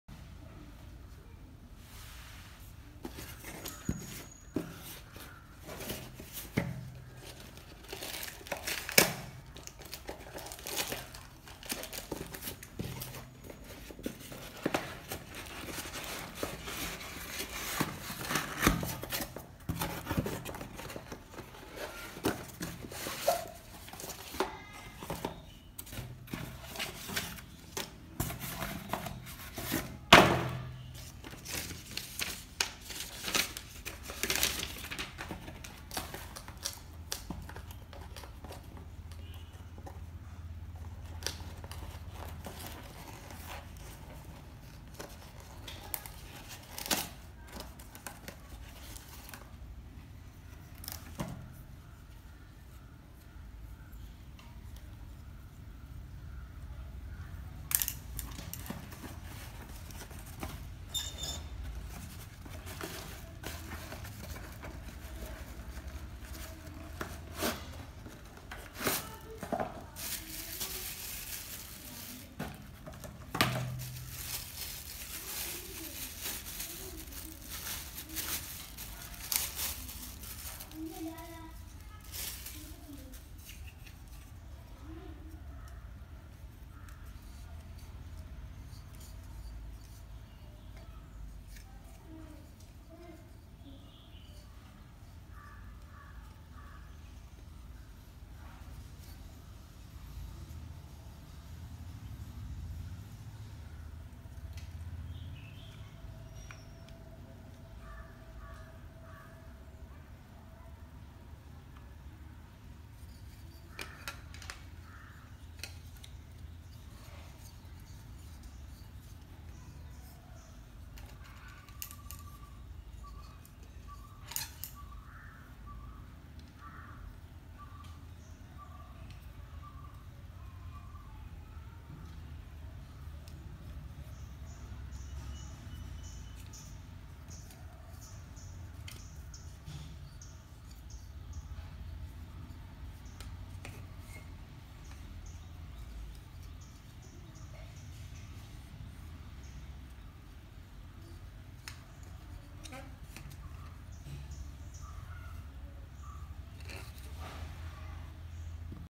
Mini Yuntung Tripod
Unboxing of mini tripod
Raptas Mini tripod YT-228 universal Mount Phone Holder